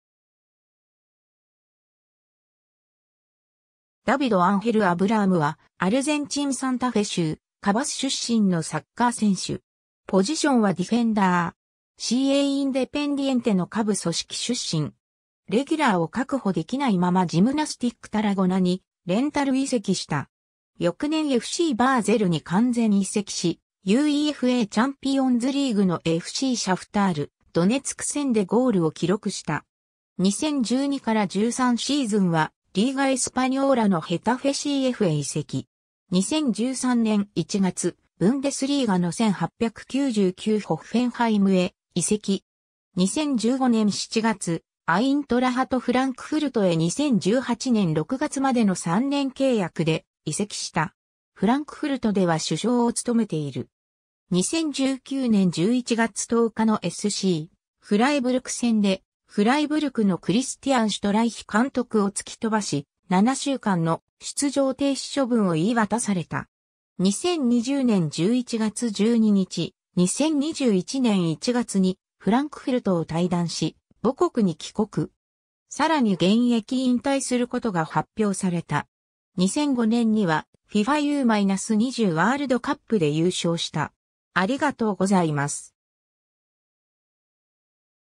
ダビド・アブラーム
ジムナスティック・タラゴナの選手
ダビド・アンヘル・アブラーム（David Ángel Abrahám, 1986年7月15日 - ）は、アルゼンチン・サンタフェ州カバス出身のサッカー選手。
 CAインデペンディエンテの下部組織出身。
レギュラーを確保できないままジムナスティック・タラゴナにレンタル移籍した。
翌年FCバーゼルに完全移籍し、UEFAチャンピオンズリーグのFCシャフタール・ドネツク戦でゴールを記録した。
 2012-13シーズンはリーガ・エスパニョーラのヘタフェCFへ移籍。
 2013年1月、ブンデスリーガの1899ホッフェンハイムへ移籍。
 2015年7月、アイントラハト・フランクフルトへ2018年6月までの3年契約で移籍した。
フランクフルトでは主将を務めている。
 2019年11月10日のSCフライブルク戦でフライブルクのクリスティアン・シュトライヒ監督を突き飛ばし、7週間の出場停止処分を言い渡された。
 2020年11月12日、2021年1月にフランクフルトを退団し、母国に帰国。
更に現役引退することが発表された。
 2005年にはFIFA U-20ワールドカップで優勝した。